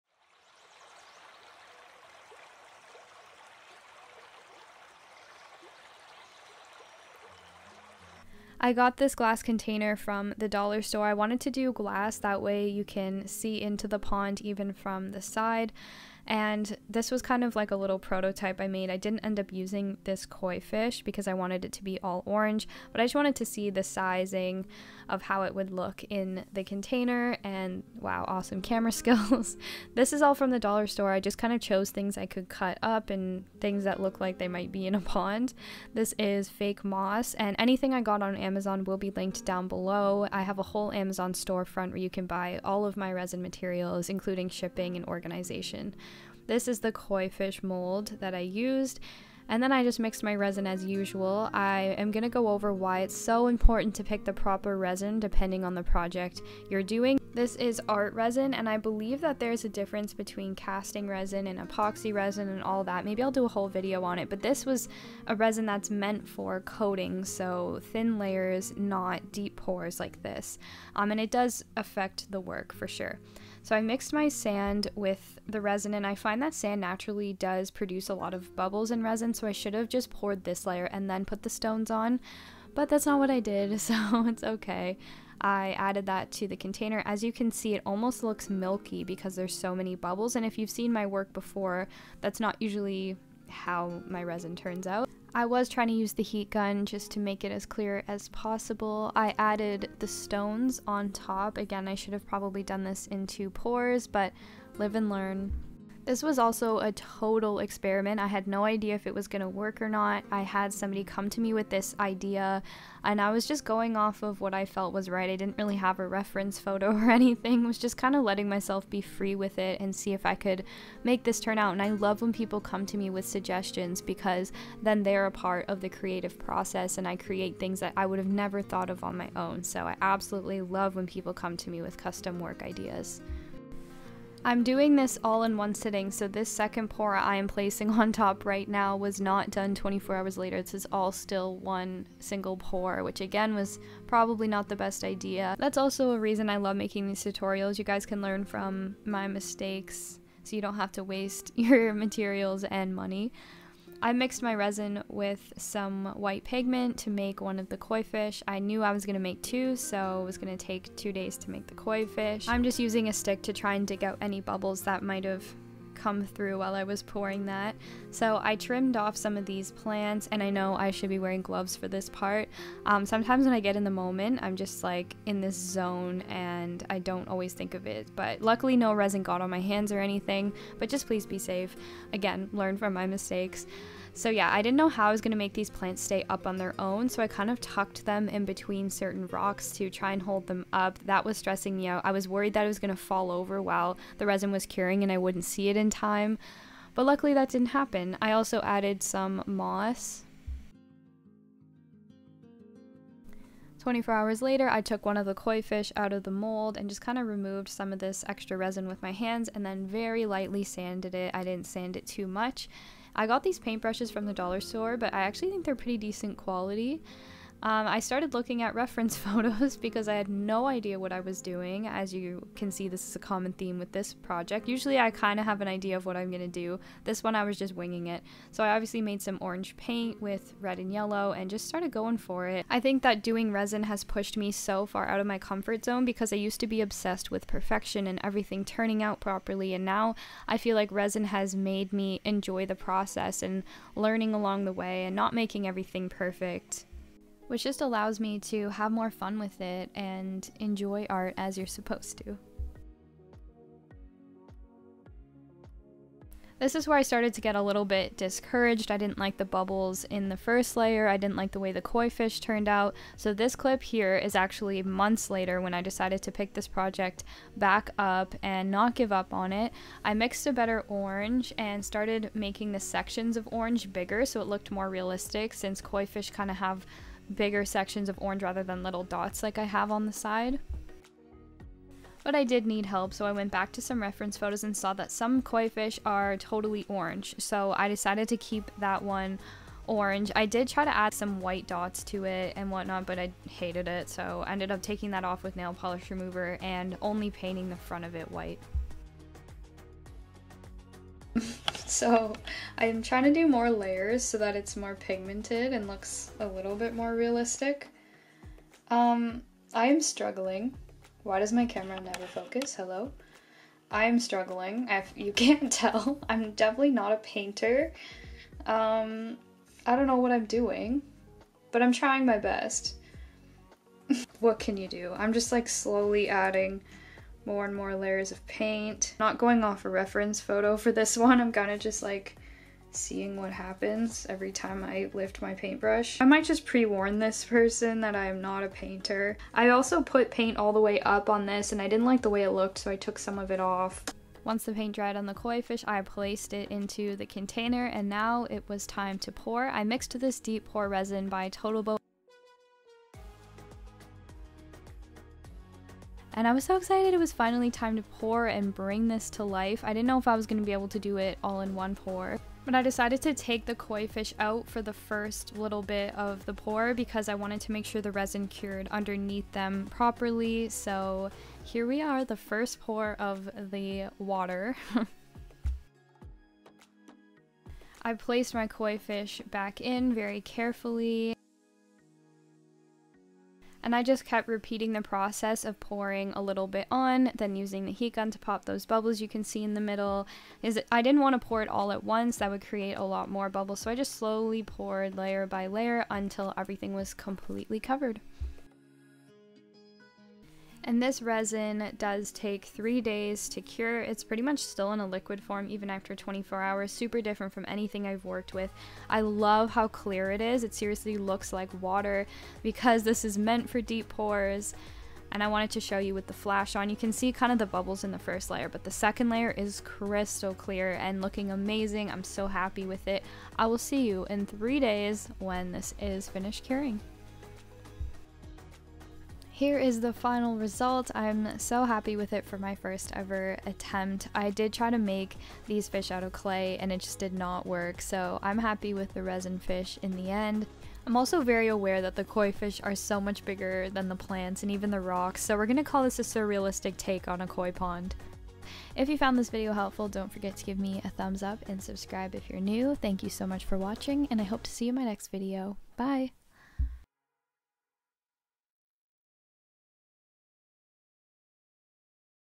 How I Made a Resin Koi Fish Pond | Koi Pond Tutorial
Here is an experimental process on how I made a resin koi fish pond! I actually loved how this turned out and I think it was a great first attempt!

❤️FOLLOW ME❤️

(Also available on Anchor and Spotify under “Dion Girls”)

MEG DION
NEWMARKET ON L3X 1Y8
CANADA